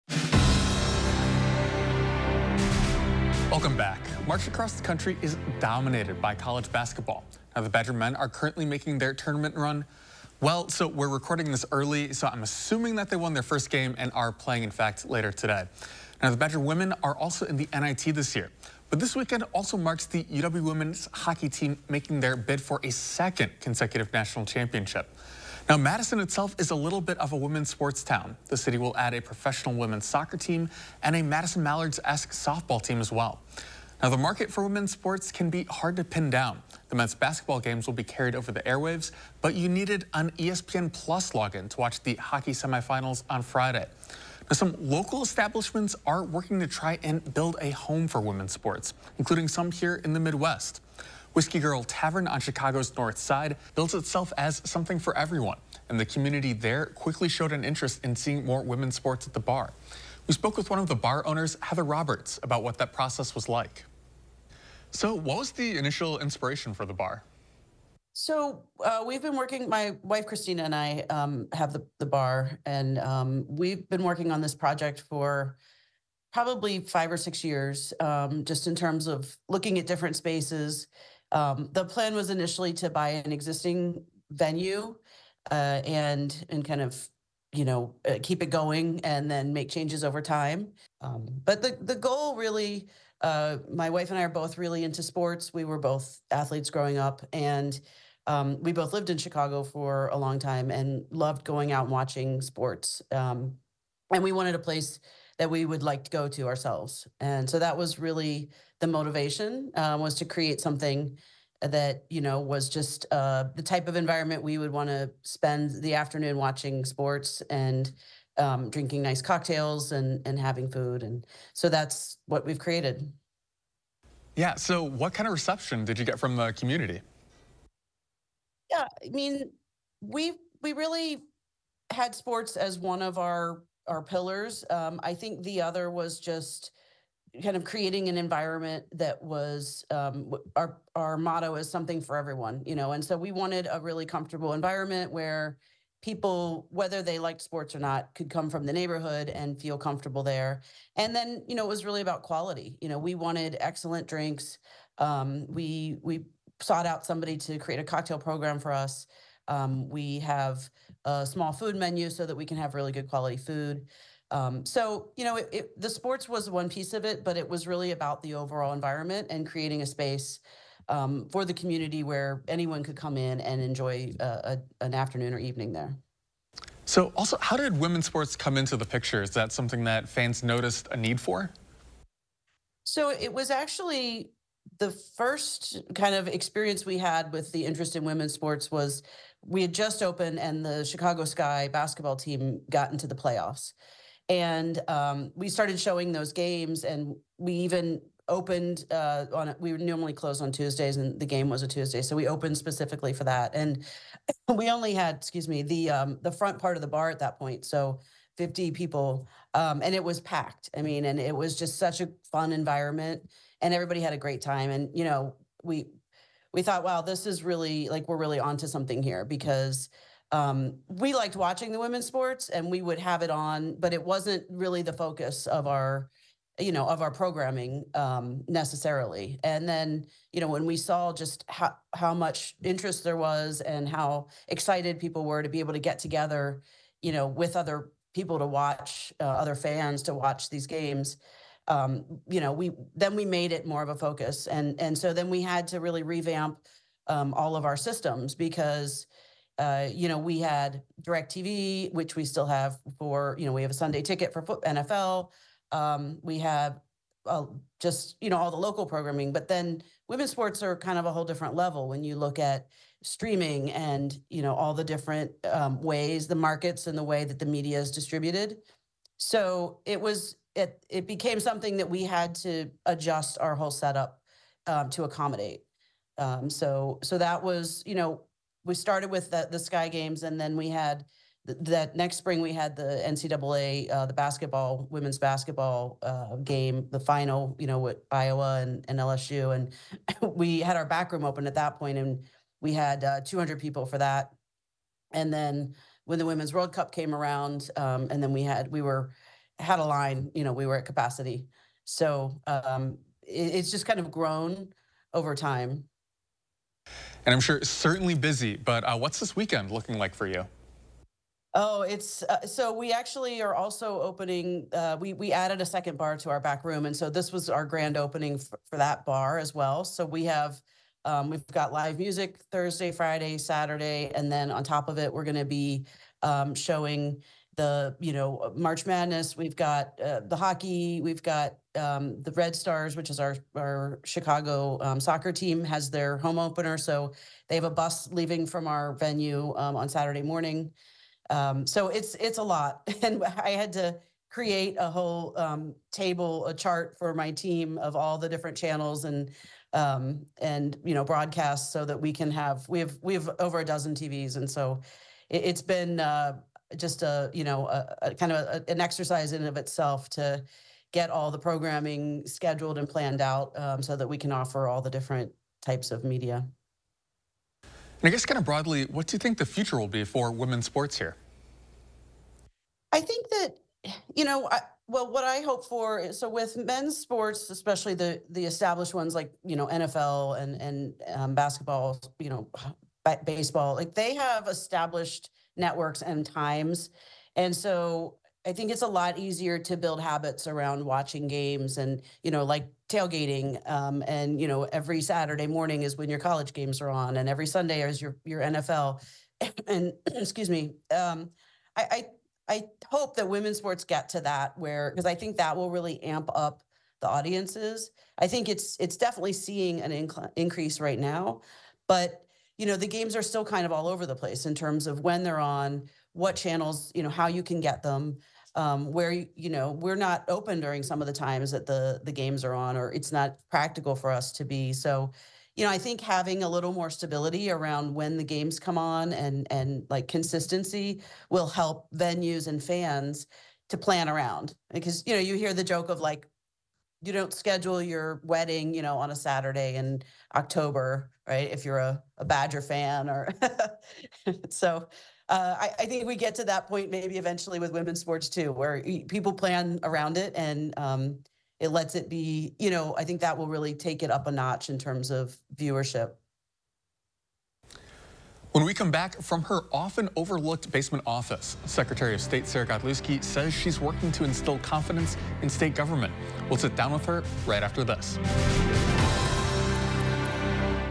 For the Record: Impact of tournament season on women's sports
March is tournament season for college athletes, and women's sports have seen a boost.